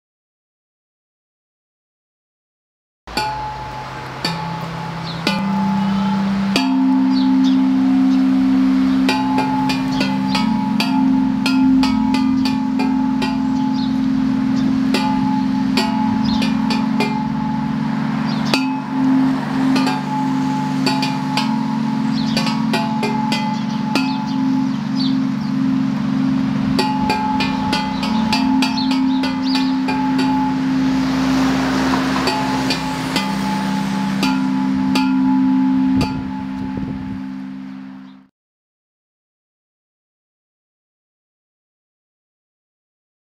Percussion Play Emperor Chimes - Outdoor Musical Instruments
With the largest standing over 2.5m in height, our Emperor Chimes really are the daddy of all chimes - large in sound and size!